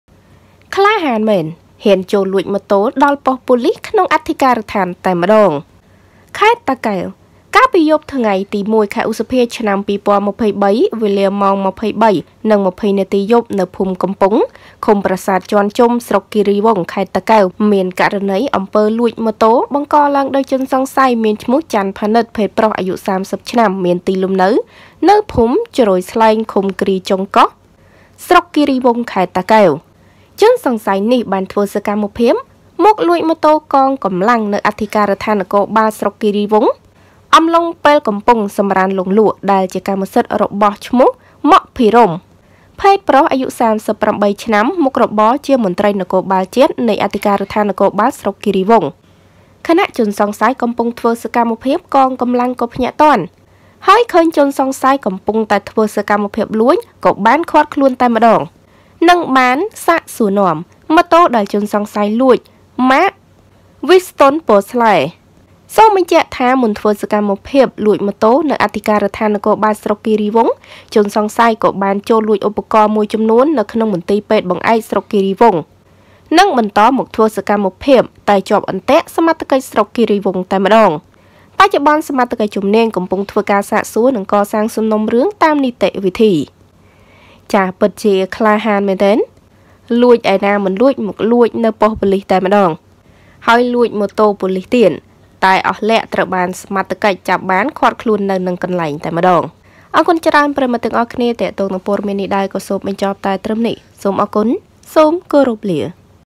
ក្លាហានមែន! ហ៊ានចូលលួចម៉ូតូប៉ូលិសដល់ក្នុងអធិការដ្ឋានតែម្ដងចុងក្រោយ..
សូមអធ្យាស្រ័យ

ចំពោះការមិនបានតប ខំមិនបានគ្រប់គ្នា

Ms Socheata